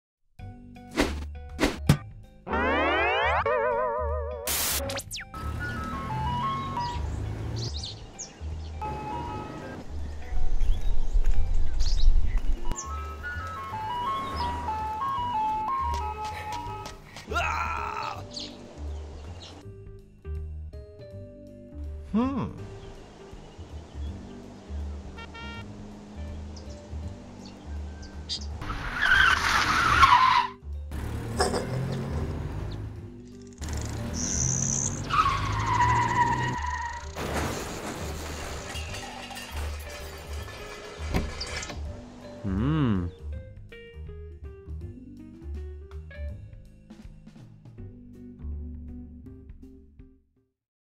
Catch A Cone
An animation by Jordan Groves. This is my final school project for 2013, I did the; modeling, texturing, lighting and animating then put everything together in Premier.